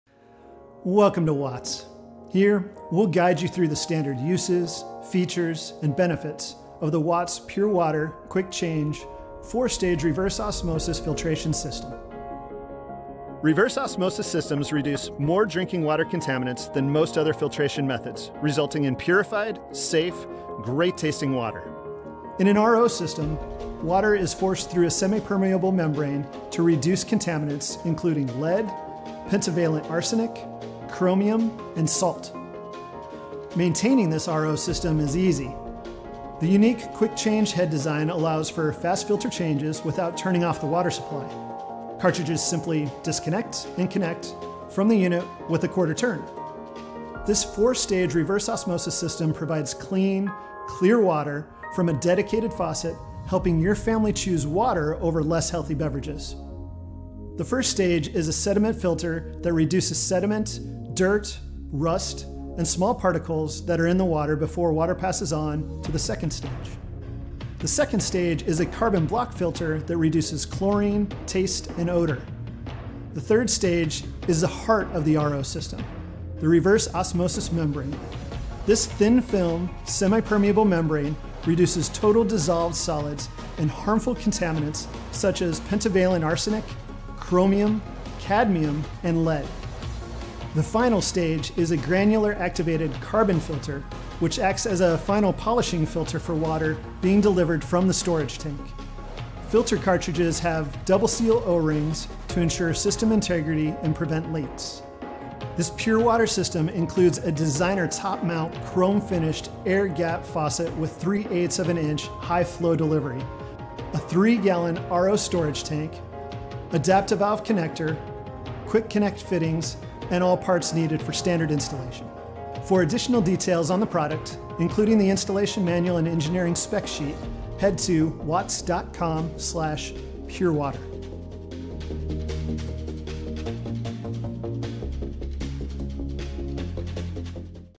Better than Bottled Water? Pure Water RO System from Watts
Discover how the Series PWROKC4 Kwik-Change RO System uses reverse osmosis to produce high-quality water that exceeds the quality of most bottled waters.